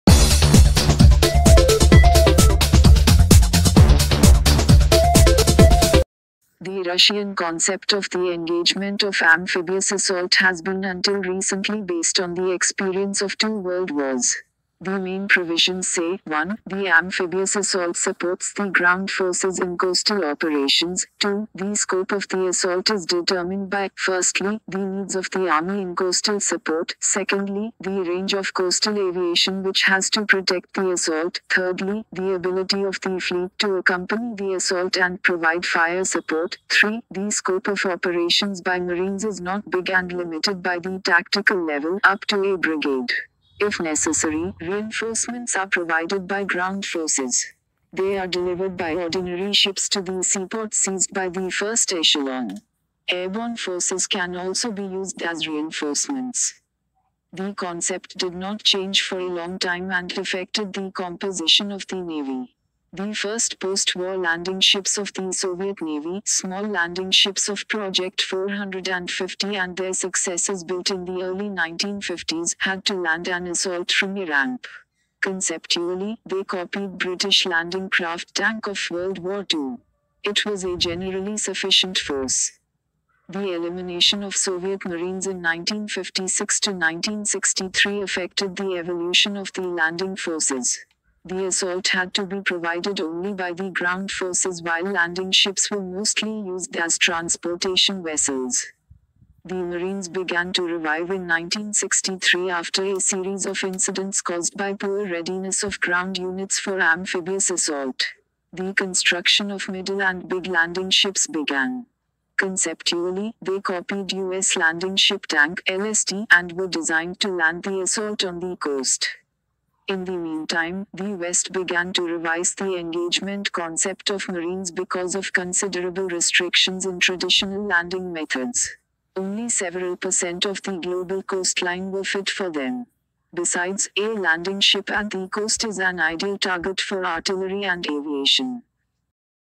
Analysis Part 1: Russian Navy starts production of amphibious assault ships Project 23900
Zvezda Shipyard in Kerch will lay two new warships shortly. The universal landing ships (UDK) of project 23900 have to join the Navy by the end of 2027 and are the most expected ships in the past fifty years of the Russian Navy. The Profile magazine writes about the development of Russian amphibious assault forces.